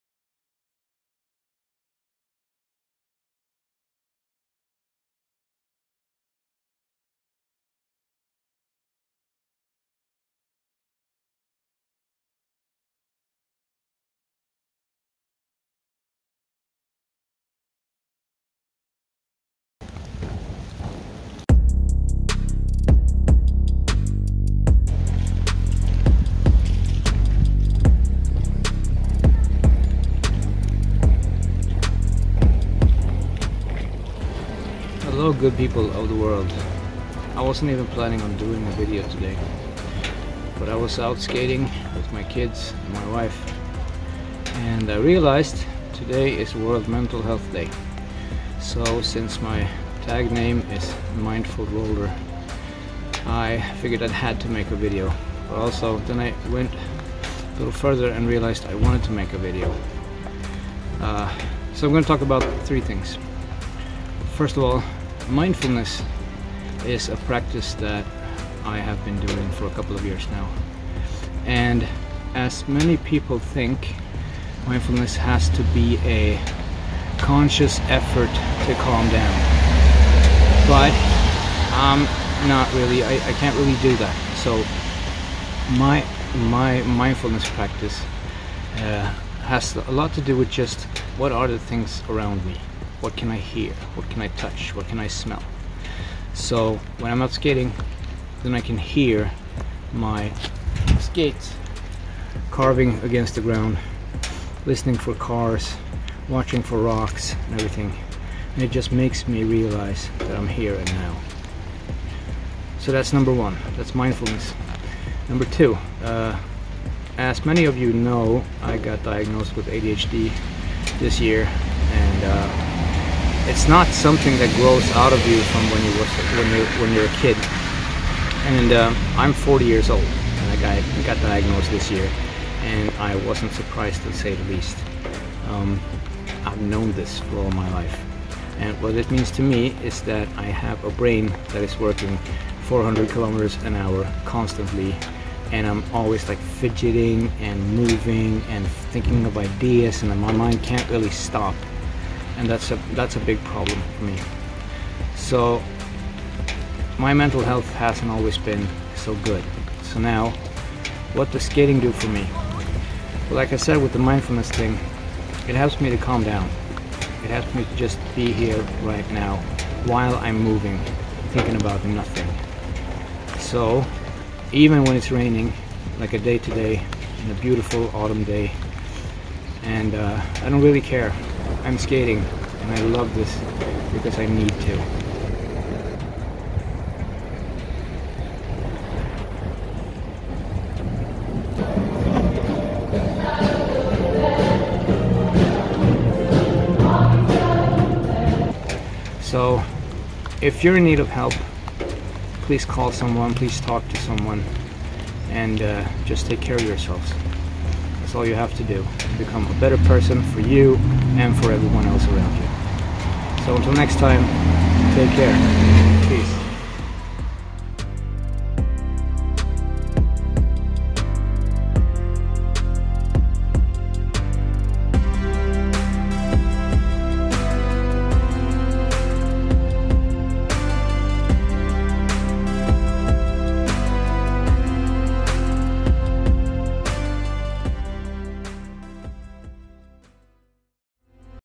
World Mental Health day - Mindfulness, ADHD and Inline Skating (rolki, patins) - Uppsala, Sweden
Long text. But please read.

Earlier this year I was diagnoed with ADHD. I wasn't the least bit surprised. I have known this since I found out adults can have it too. The difference was, now I could get legit help because I could say I have the diagnosis.

My life has been a rollercoaster of emotions, there are so many ups and downs that I can't count them and the difference from a "normal" brain is that my ups and downs depend on day, mood, hours of sleep, and they occur on a daily basis.

There's been anxiety, periods of depression, so many things gone wrong because of impulsivity and inattention.

But now I'm taking care of it. I'm taking medication that allows my mind to aim my brain for targets instead of randomly pushing them off the road.

Many people in my life has been having an even worse ride in life than myself. And nobody should be ending their life because of mental illness. These things hurt. They hurt like hell.

Please find help. It's super important that you do.

Skaters of the world unite and let's take care of each other. Let's pick each other up if we fall.

(which I forgot to mention in the video...so sorry...)